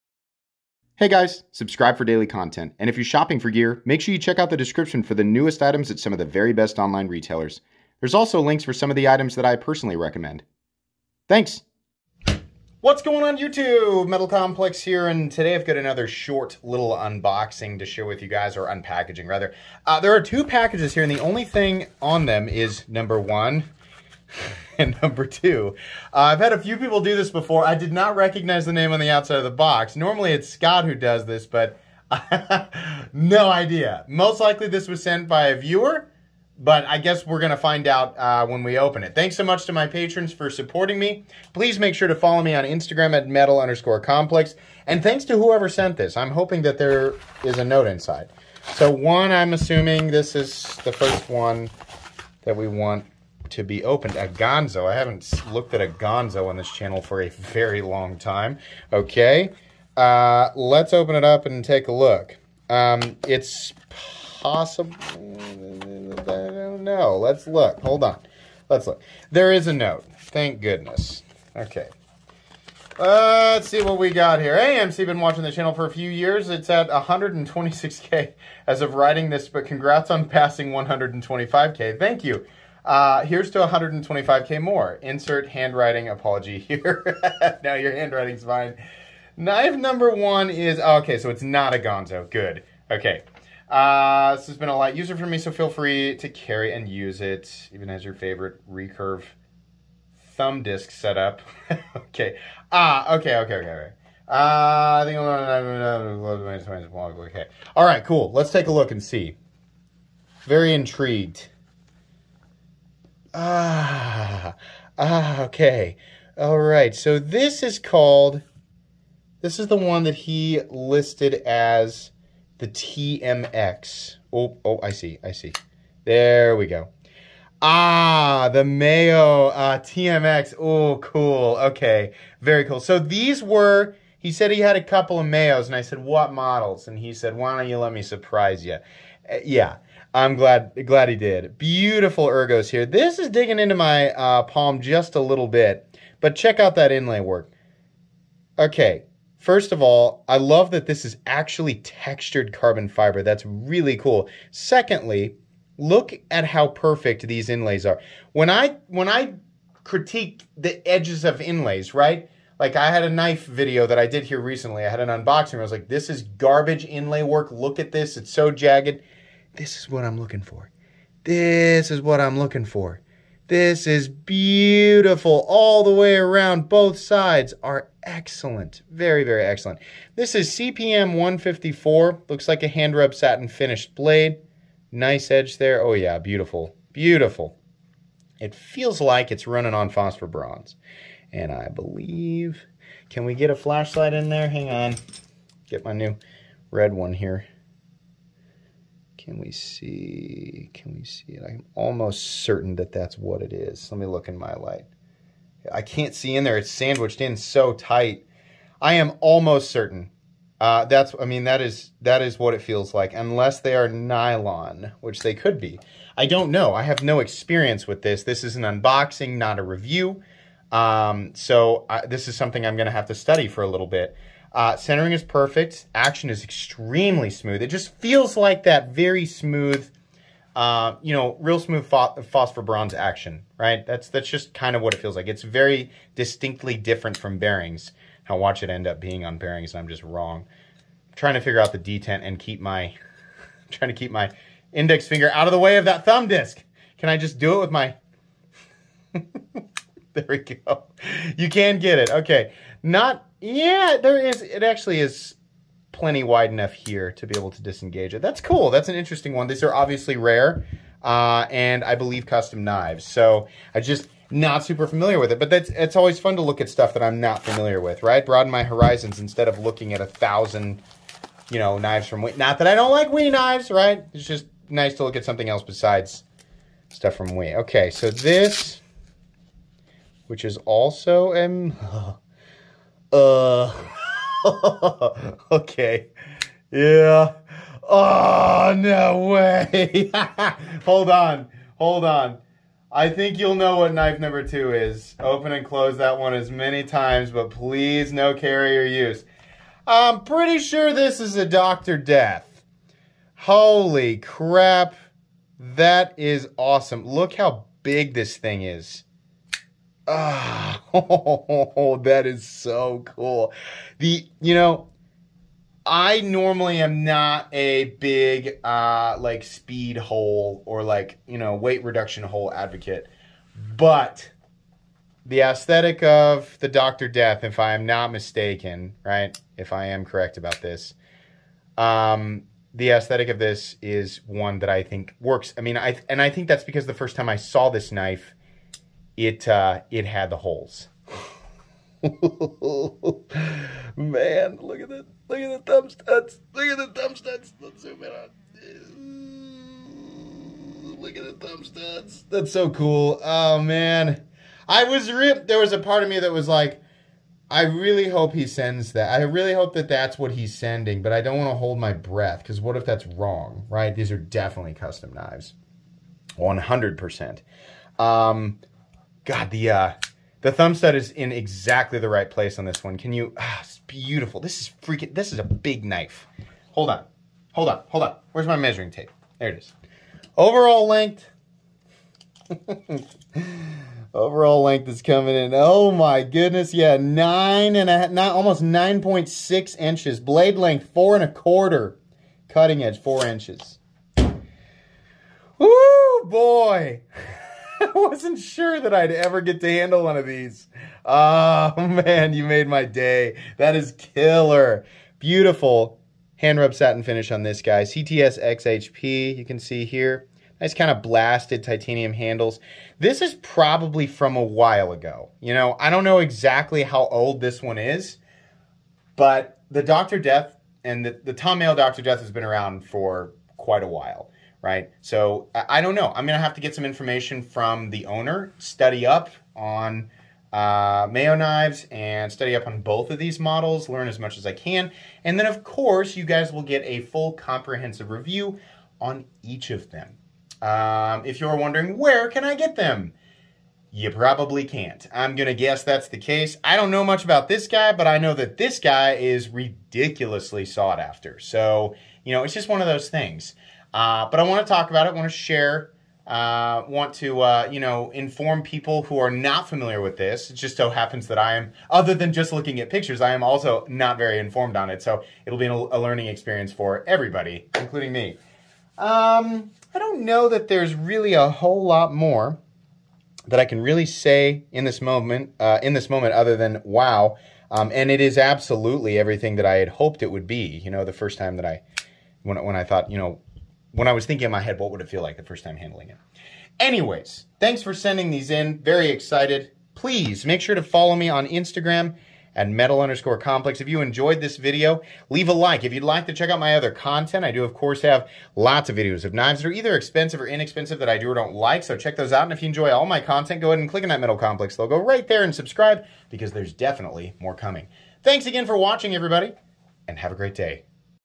The Day Has Finally Come - Can't Believe I'm Holding This! - Unboxing
Unboxing a Tom Mayo Dr. Death.